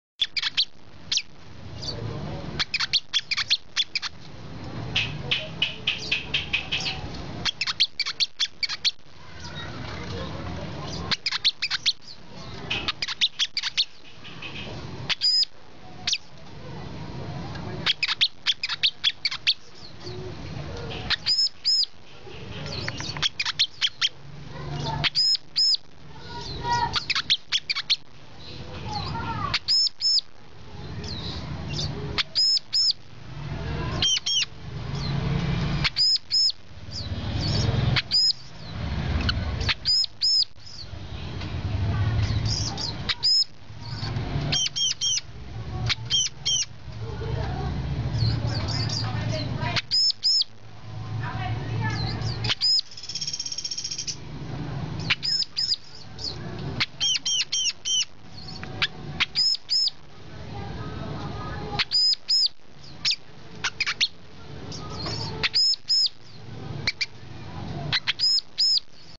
Burung Master : Kolibri Kelapa Mental Baja Anti Stres
Dikenal sebagai penetap, baik di pekarangan terbuka, perkebunan kelapa, semak pantai, dan hutan mangrove. Bersifat teritorial secara agresif, mengusir burung-madu lainnya dari pohon atau sumber makanan yang disukainya.